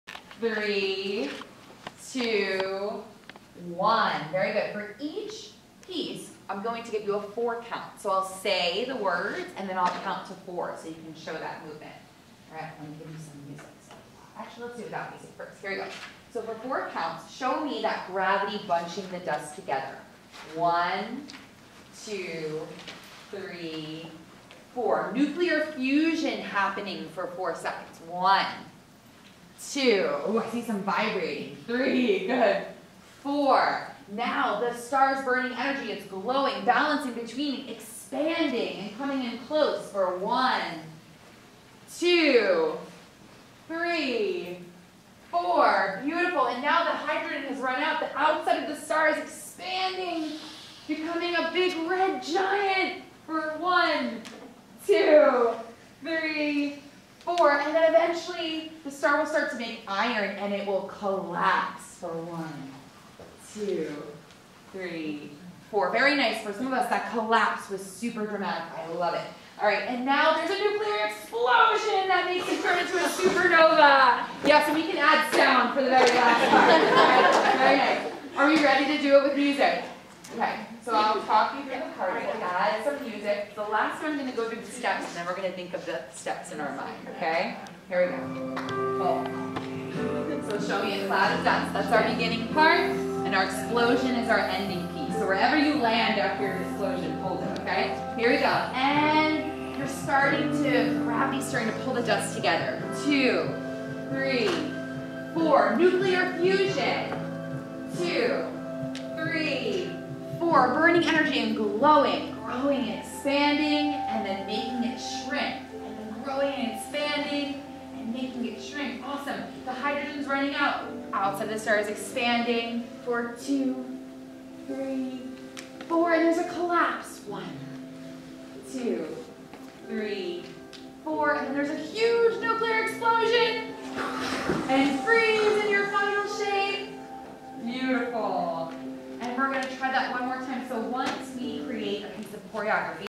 Dancing Through Space Part 2
An arts integrated approach to understanding how stars, meteorites, asteroids, and supernovas are created. This is a professional development workshop led by arts integration specialist, Jessica DiLorenzo.